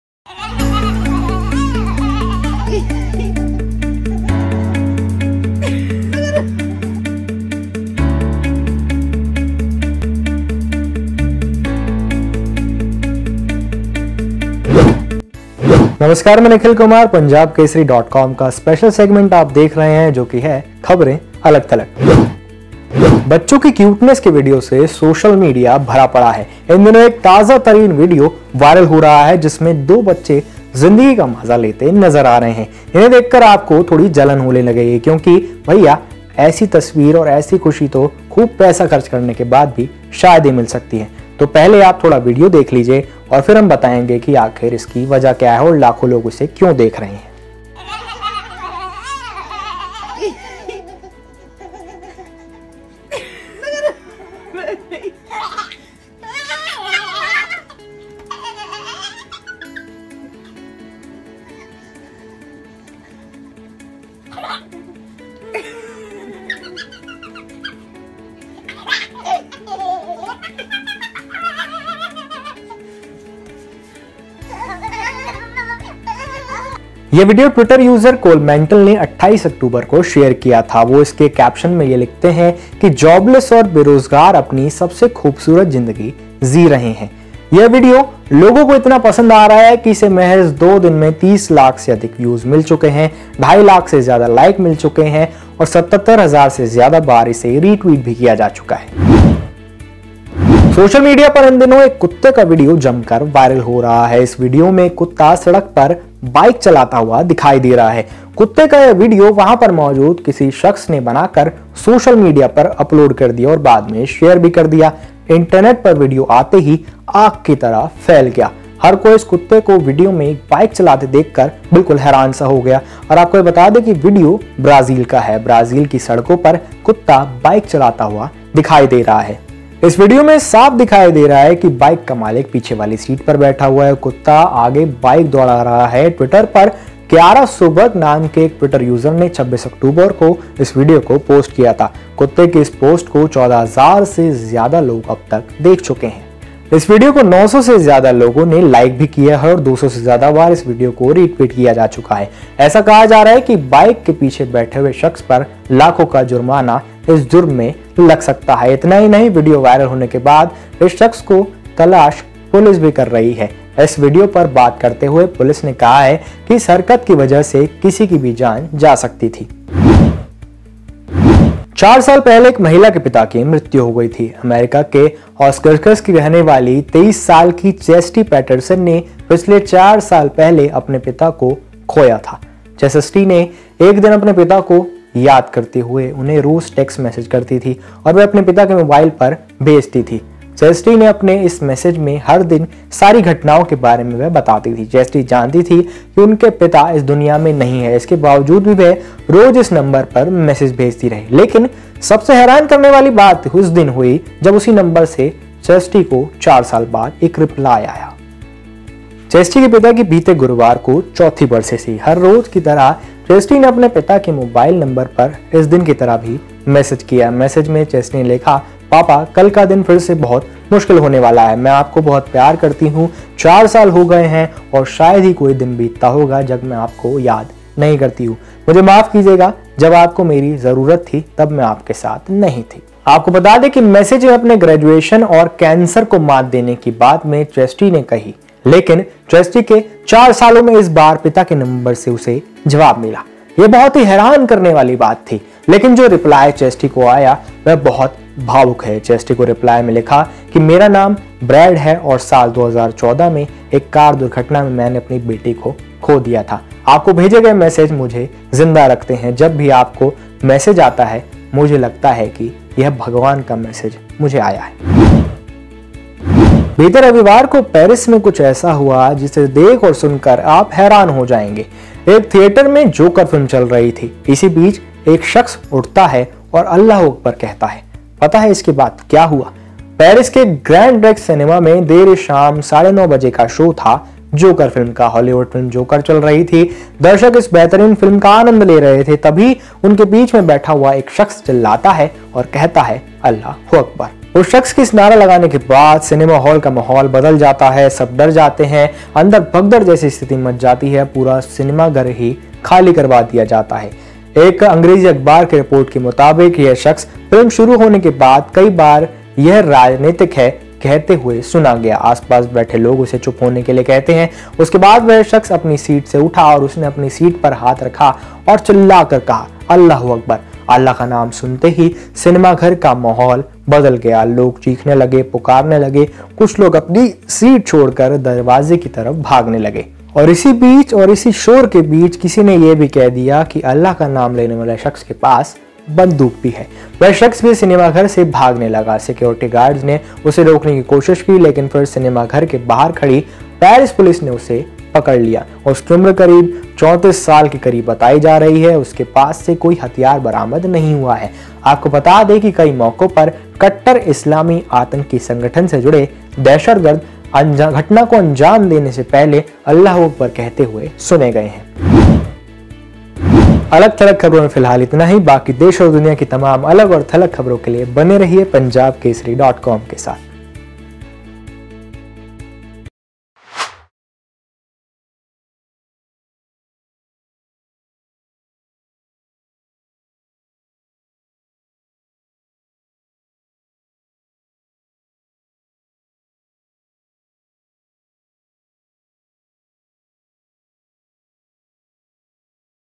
Khabre Alag Thalag - सोशल मीडिया पर कुत्ते का बाइक चलाते वीडियो हुआ वायरल, लोगों ने दिए ऐसे रिएक्शन
साेशल मीडिया पर इन दिनों एक कुत्ते का वीडियो जमकर वायरल हो रहा है। इस वीडियो में एक कुत्ता सड़क पर बाइक चलाता हुआ दिखाई दे रहा है। कुत्ते का यह वीडियो वहां पर मौजूद किसी शख्स ने बनाकर सोशल मीडिया पर शेयर कर दिया। इंटरनेट पर वीडियो आते ही आग की तरह फैल गया। हर कोई कुत्‍ते को वीडियो में बाइक चलाते देखकर हैरान है। बता दें कि यह वीडियो ब्राजील का है।